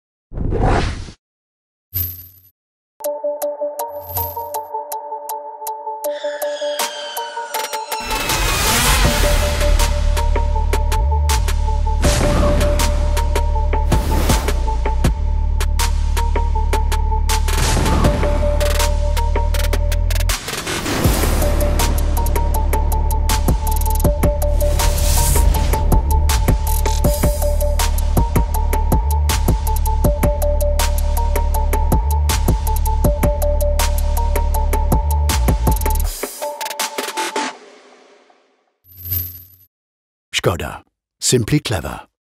Skoda Vision RS: así será el nuevo compacto de Skoda en 2019 | Autopista.es
Skoda desvela en el Salón de París el prototipo Vision RS, un modelo que anticipa casi por completo al próximo sustituto del Spaceback. El nuevo compacto llegará al mercado a lo largo de 2019.